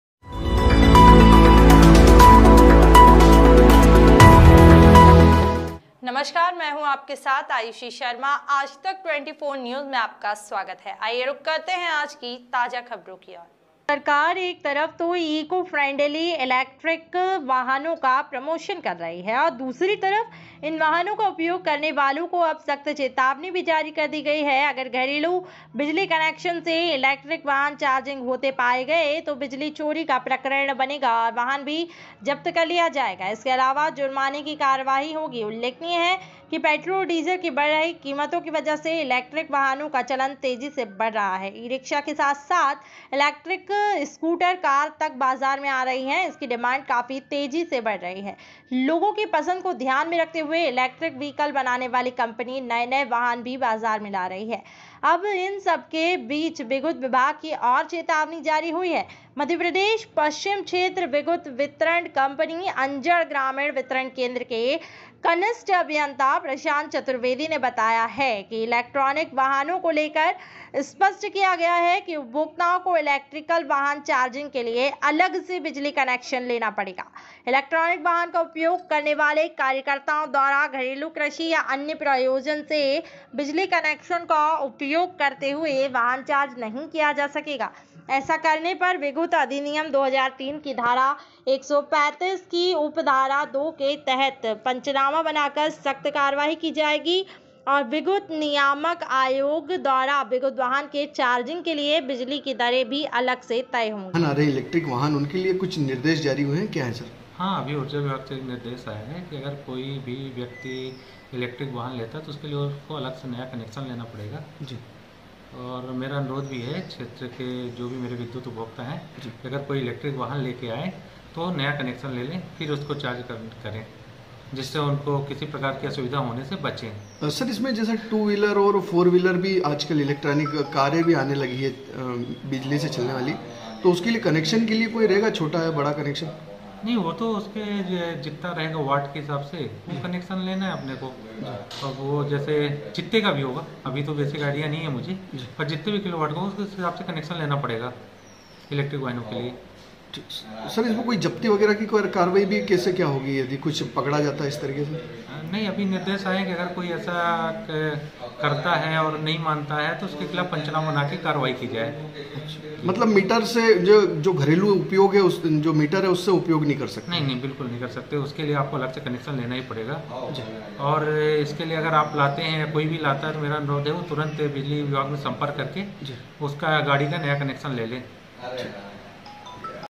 जिले में दौड़ रहे बिना पेट्रोल-डीजल के वाहन, अब इने लेना होगा अलग से बिजली कलेक्शन वरना
अंजड़

स्लग :- *जिले में दौड़ रहे बिना पेट्रोल-डीजल के वाहन, अब इने लेना होगा अलग से बिजली कलेक्शन वरना होगी कार्रवाई*

एंकर :- सरकार एक तरफ तो इको फ्रेंडली इलेक्ट्रिक वाहनों का प्रमोशन कर रही है, वहीं दूसरी तरफ इन वाहनों का उपयोग करने वालों को अब सख्त चेतावनी भी जारी कर दी गई है. अगर घरेलू बिजली कनेक्शन से इलेक्ट्रिक वाहन चार्जिंग होते पाया गया, तो बिजली चोरी का प्रकरण बनेगा और वाहन भी जब्त कर लिया जाएगा. इसके अलावा जुर्माने की कार्यवाही भी होगी. उल्लेखनीय है कि पेट्रोल डीजल के बढ़ रही कीमतों की वजह से इलेक्ट्रिक वाहनों का चलन भी तेजी से बढ़ रहा है. ई-रिक्शा के साथ-साथ इलेक्ट्रिक स्कूटर, कार तक बाजार में आ रही है. इनकी डिमांड भी काफी तेजी से बढ़ रही है. लोगों की पसंद को ध्यान में रखते हुए इलेक्ट्रिक व्हीकल बनाने वाली कंपनियां नए-नए वाहन भी बाजार में ला रही है,अब इस सबके बीच विघुत विभाग की ओर से चेतावनी जारी की गई है। मध्यप्रदेश पश्चिम क्षेत्र विघुत वितरण कंपनी अंजड ग्रामीण (2) वितरण केंद्र के कनिष्ठ अभियंता प्रशांत चतुर्वेदी ने बताया कि इलेक्ट्रॉनिक वाहनों को लेकर स्पष्ट किया गया है कि उपभोक्ताओं को इलेक्ट्रिक वाहन चार्जिंग के लिए अलग से बिजली कनेक्शन लेना पड़ेगा. इलेक्ट्रिक वाहन का उपयोग करने वाले उपयोगकर्ता द्वारा घरेलू कृषि या अन्य प्रयोजन से बिजली कनेक्शन का उपयोग करते हुए वाहन चार्ज नहीं किया जा सकेगा,ऐसा करने पर विद्युत अधिनियम 2003 की धारा 135 की उप धारा 2 के तहत पंचनामा बनाकर सख्त कार्रवाई की जाएगी,विद्युत नियामक आयोग द्वारा विद्युत वाहन के चार्जिंग के लिए बिजली की दरें भी अलग से तय होगी है।

बाईट :- *कनिष्ठ अभियंता प्रशांत चतुर्वेदी*

आज तक 24 न्यूज़ चैनल के लिए जिला चीफ ब्यूरो शकील मंसूरी की रिपोर्ट